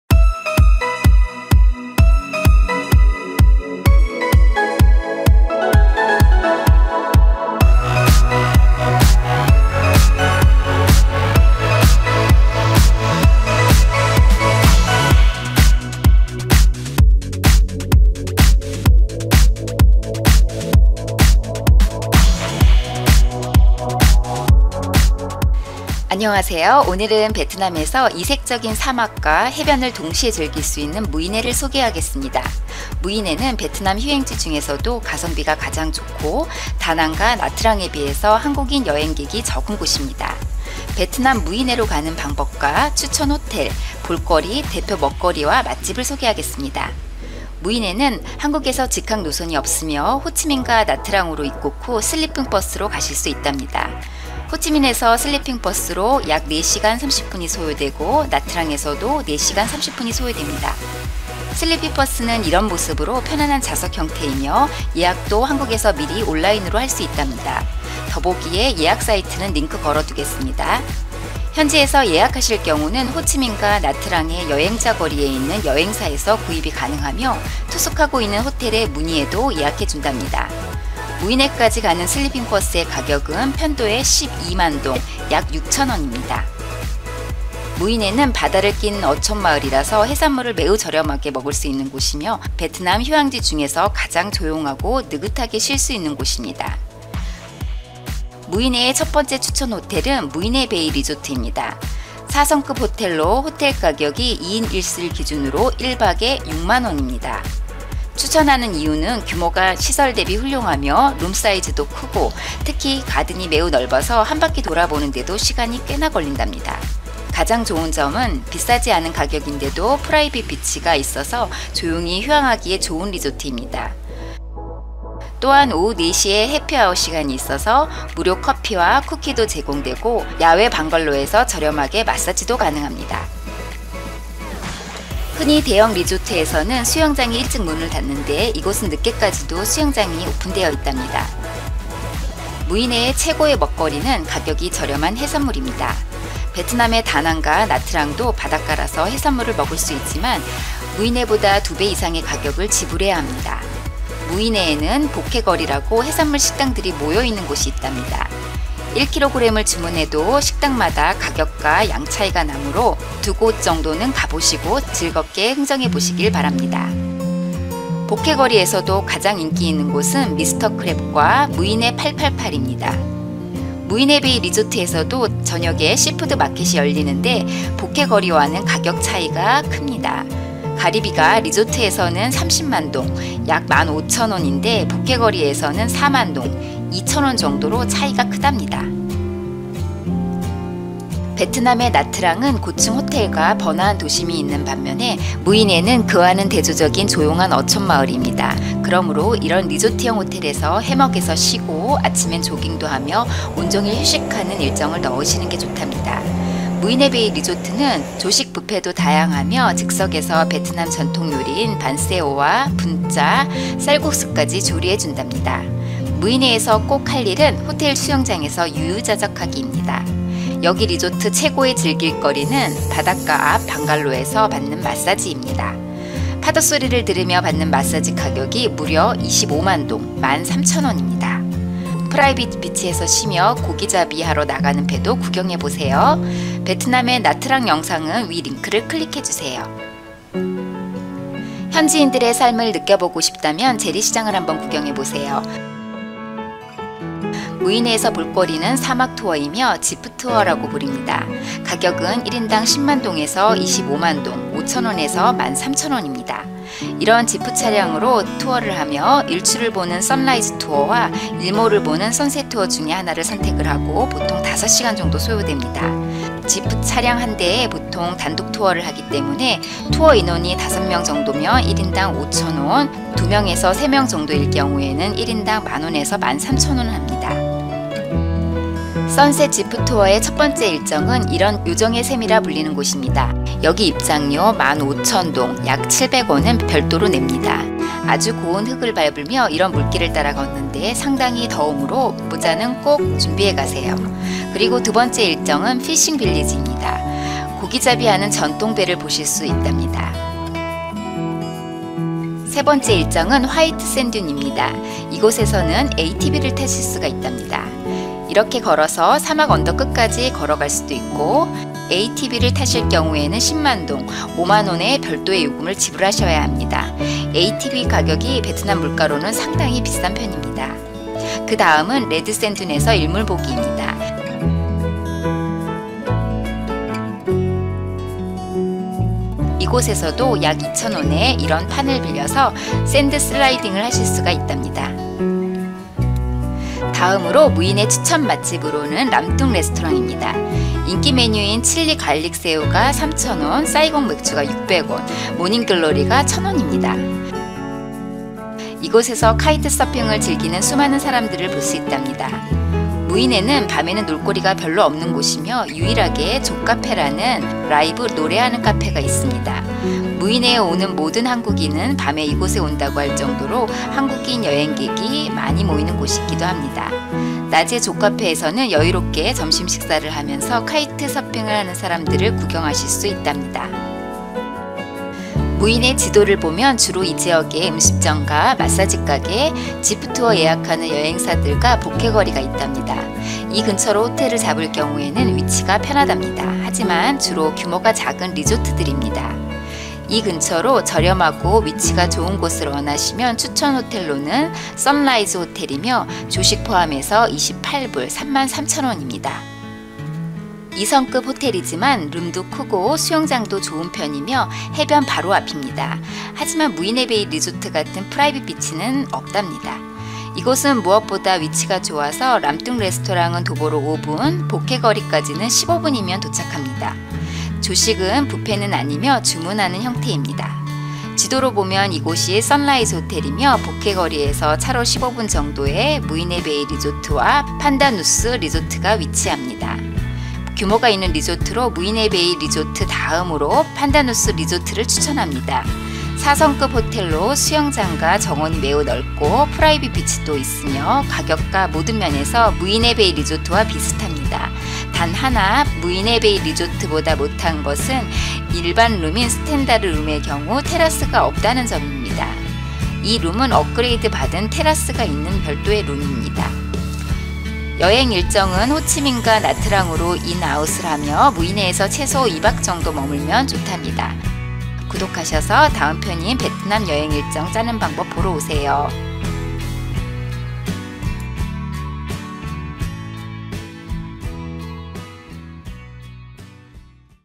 베트남의 사막 무이네 지프투어 - 슬리핑 버스 예약, 무이네 리조트 추천 3곳, 무이네 여행 정보
슬리핑 버스 예약은 신투어 리스트 온라인 예약  or 현지에서 호치민,나트랑 여행자거리의 여행사에서 예약 Tham Han Travel ,신투어 리스트등. 호텔에서도 가능.